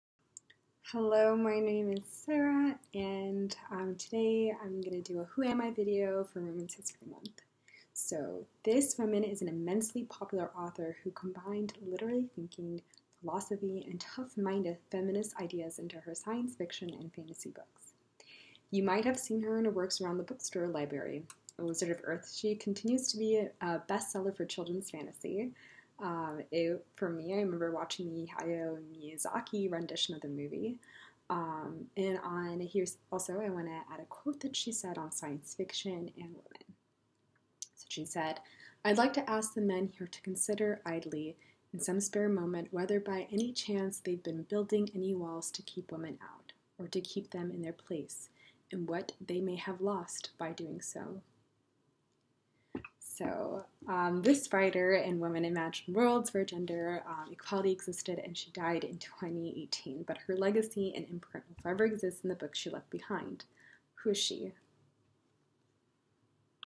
Day Four | Who Am I?
We have Ms. Sarah sharing a Woman Historical figure for the 4th day of Women's History Month! Ms. Sarah will be sharing the life, achievements and overall initiatives of this particular figure.

McClure Panthers, can you figure out who Ms. Sarah is talking about?
Answer: Ursula Le Guin